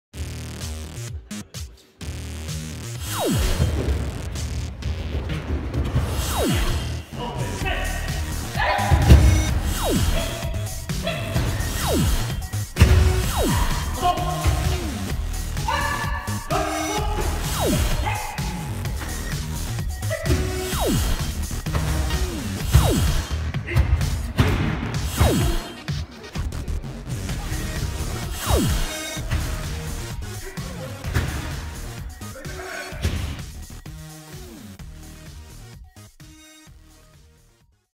Munich ENBU 2016
It was a great celebration In conjunction with Nagano Shihan birthday , it was an honour to witness aikido-ka from all over Europe get together with Unbreakable spirit and bonding  to share and promote this beautiful art.